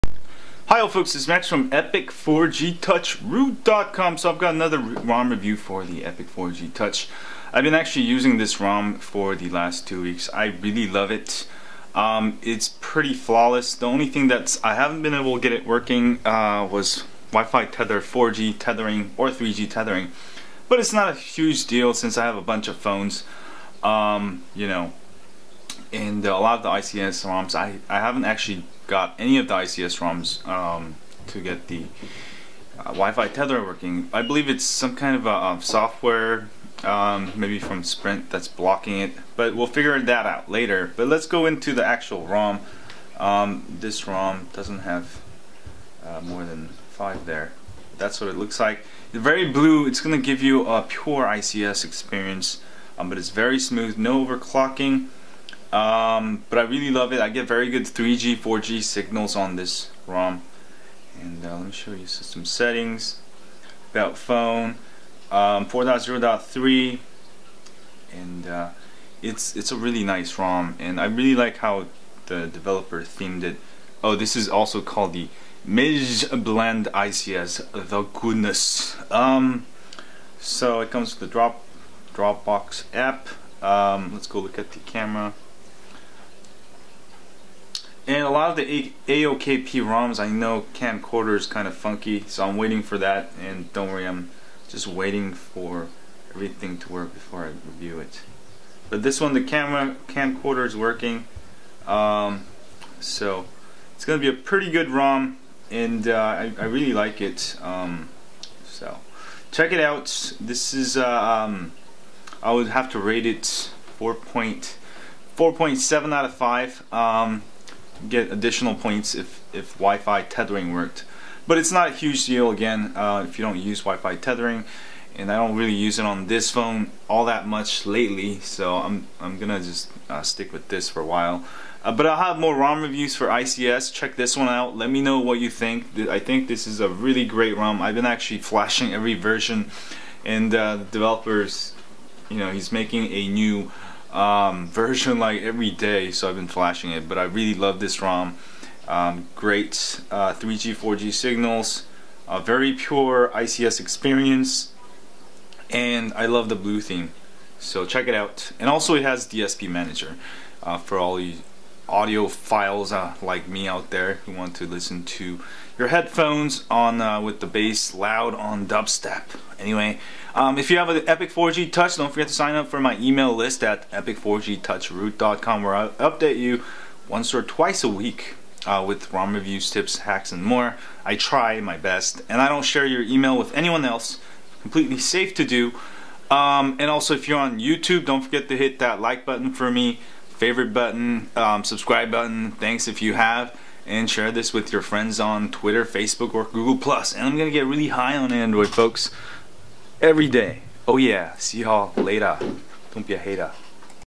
MIJJz Blend ICS ROM for Epic 4G Touch!
Here's a quick overview of MIJJz Blend ICS ROM for Epic 4G Touch!

Love this ROM!

Want to stay updated on all Epic 4G Touch Root tips and hacks?

Epic 4G Touch Reference
You will need a rooted Epic 4G Touch to install all ROM/kernels.

Sprint Epic 4G Touch SPH-D710, Please see FAQ here for all rooting, backup, install guides: